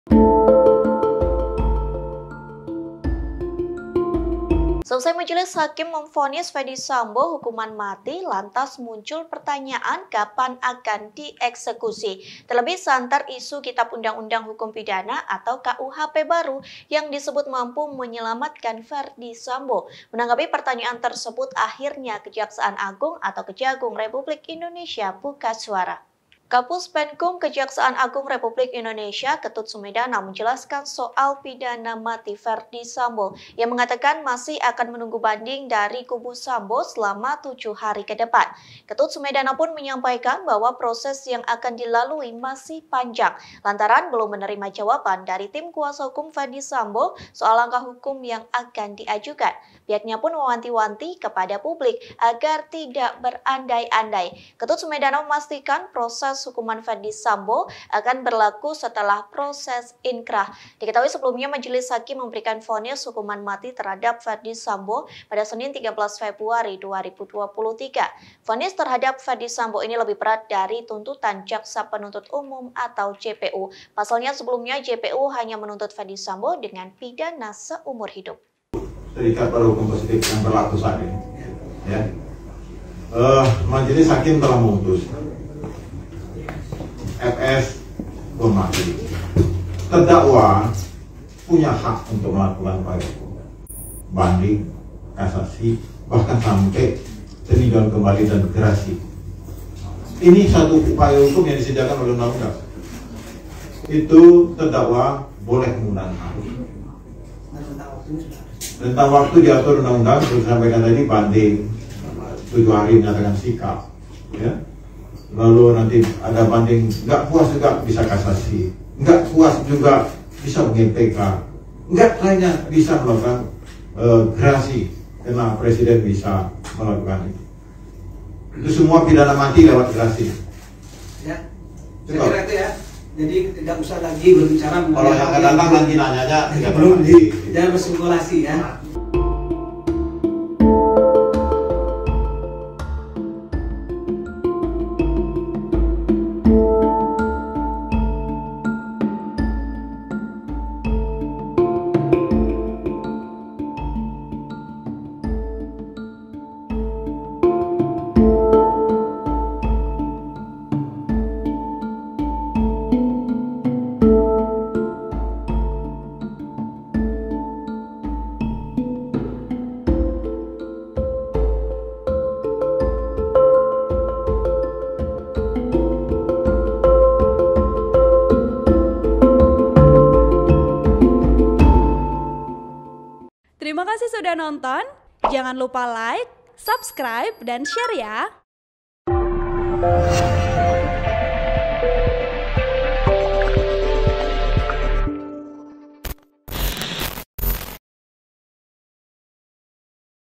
Kejagung RI Jawab Kapan Ferdy Sambo Dieksekusi Mati, Proses yang Dilalui Masih Panjang
SURYAMALANG.COM - Seusai Majelis Hakim memvonis Ferdy Sambo hukuman mati, lantas muncul pertanyaan kapan akan dieksekusi?

Terlebih santer isu Kitab Undang-Undang Hukum Pidana (KUHP) baru dapat yang disebut mampu selamatkan Ferdy Sambo.

Menanggapi pertanyaan tersebut, akhirnya Kejaksaan Agung (Kejagung) RI buka suara.

Kapuspenkum Kejaksaan Agung RI Ketut Sumedana menjelaskan soal pidana mati Ferdy Sambo.

Ia mengatakan masih akan menunggu banding dari kubu Sambo selama 7 hari ke depan.

Ketut Sumedana pun menyampaikan bahwa proses yang akan dilalui masih panjang.

Lantaran belum menerima jawaban dari tim kuasa hukum Ferdy Sambo soal langkah hukum yang akan diajukan.

Pihaknya pun mewanti-wanti kepada publik agar tidak berandai-andai.

Ketut Sumedana memastikan proses hukuman Ferdy Sambo akan berlaku setelah proses inkrah.

"Jadi kita menunggu proses jangan berandai-andai. Bagaimana prosesnya (eksekusi) nanti, nanti setelah prosesnya inkrah," tukasnya.

Diketahui sebelumnya, Majelis Hakim memberikan vonis hukuman mati terhadap Ferdy Sambo pada Senin (13/2/2023).

Vonis terhadap Ferdy Sambo ini lebih berat dari tuntutan jaksa penuntut umum (JPU).

Pasalnya sebelumnya JPU hanya menuntut Ferdy Sambo dengan pidana seumur hidup. (Tribun-Video.com/Wartakotalive)

Artikel ini telah tayang di Tribunnews.com dengan judul Divonis Hukuman Mati, Kapan Jaksa Eksekusi Ferdy Sambo? Ini Penjelasan Kejagung RI,